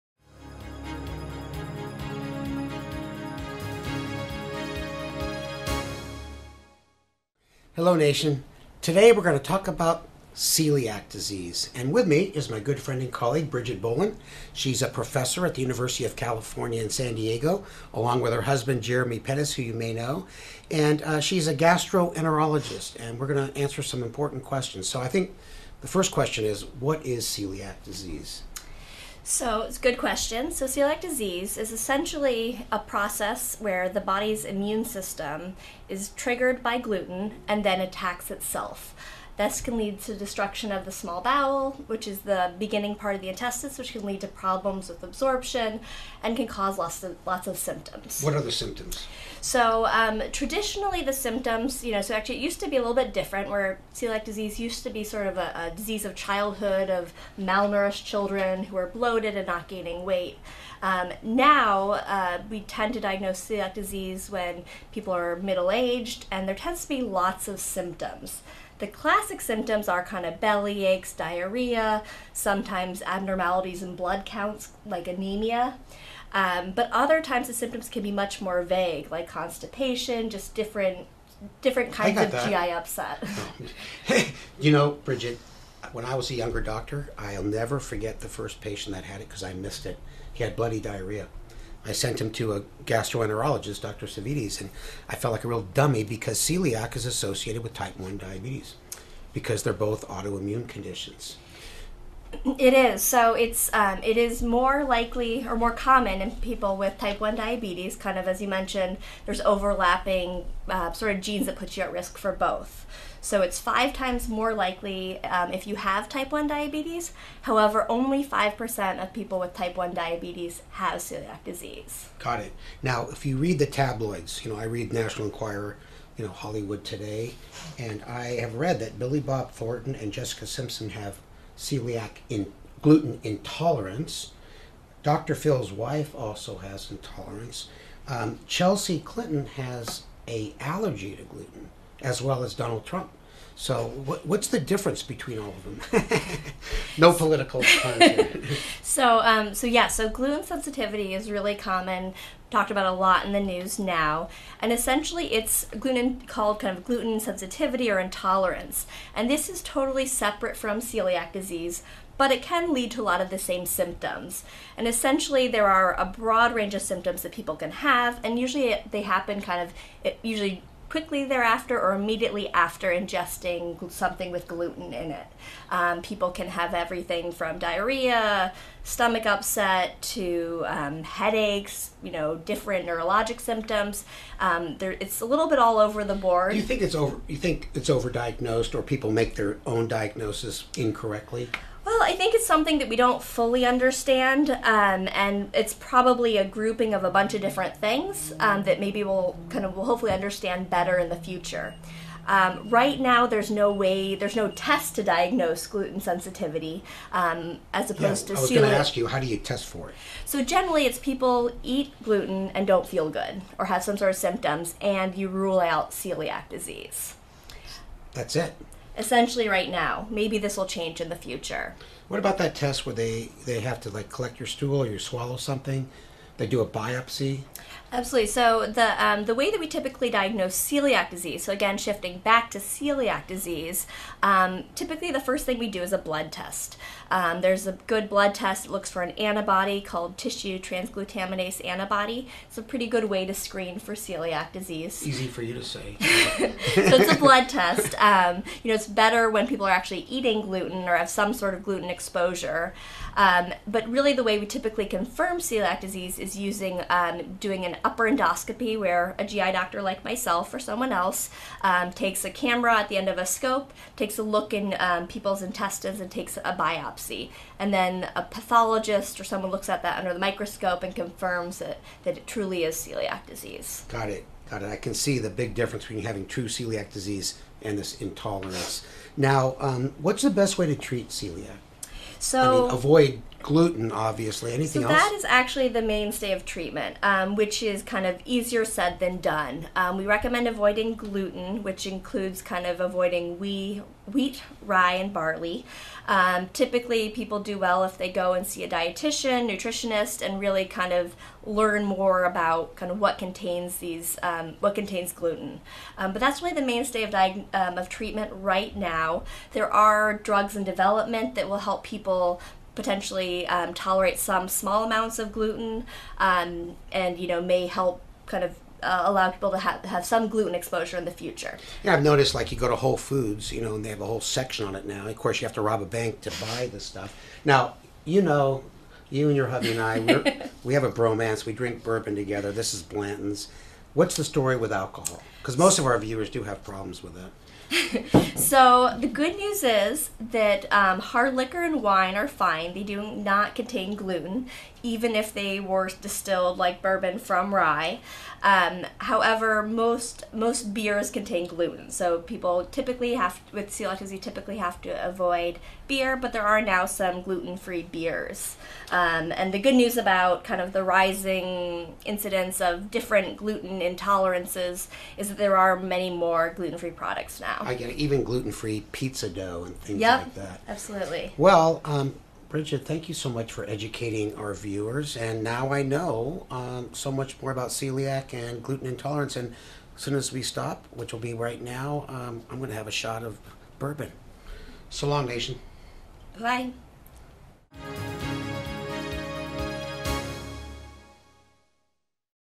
Diabetes and Celiac Disease
Gluten sensitivity seems to be a growing issue, but many people are still confused about what gluten actually is and how it could affect them if they have diabetes.  Gastroenterologist Brigid Boland explains the different types of gluten sensitivity, symptoms to look out for, and treatment options available.